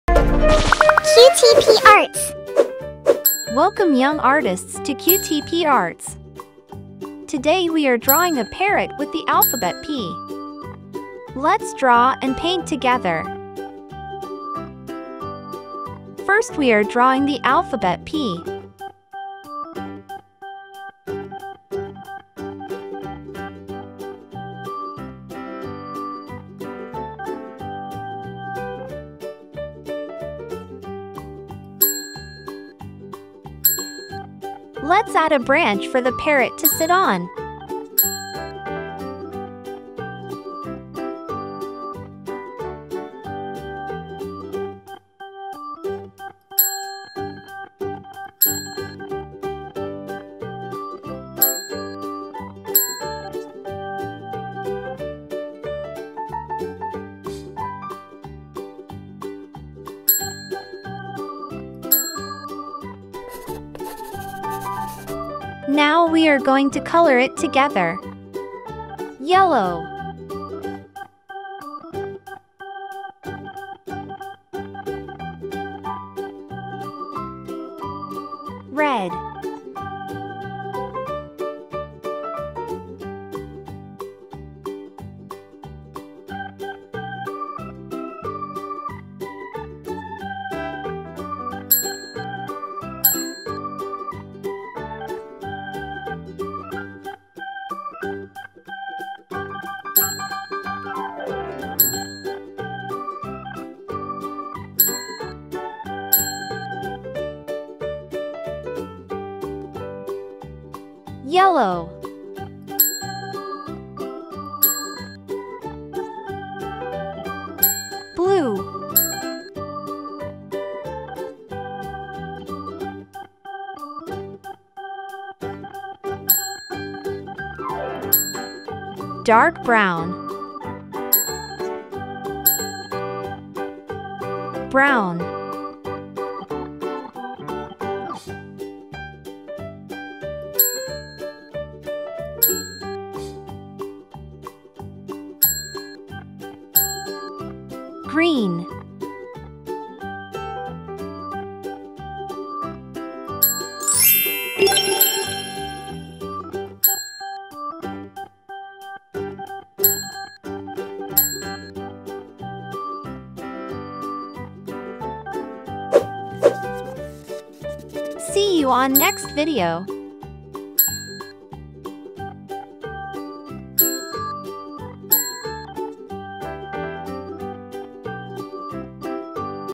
P for Parrot | How to Draw Easy Step by Step
Welcome Young Artists to QTP ARTS ! We all love Birds Specially when its hand painted and coloured.  You can draw and paint Parrot with me. Let's Paint together.

Materials:
Paper
Pencil
Sketch
Watercolour